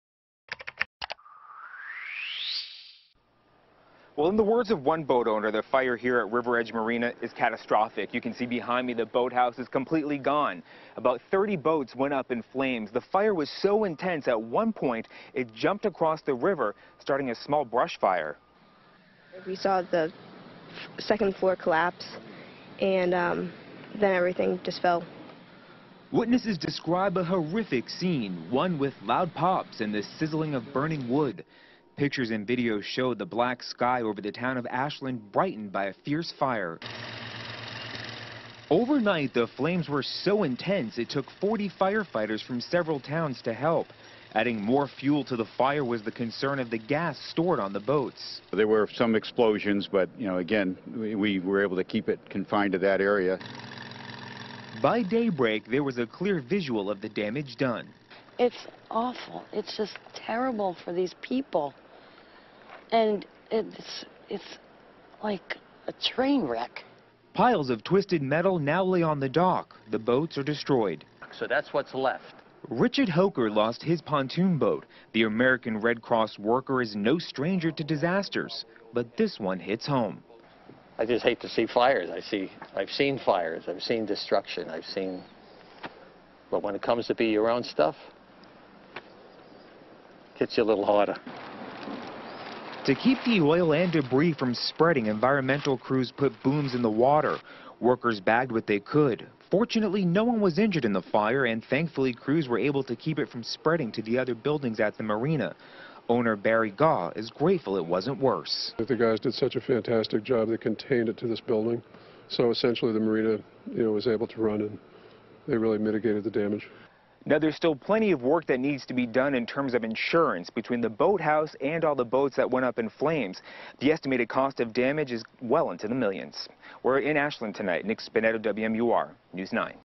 'Catastrophic' fire destroys boats at marina
Dozens of boats were destroyed in an overnight fire at a Lakes Region marina, and investigators are trying to determine the cause.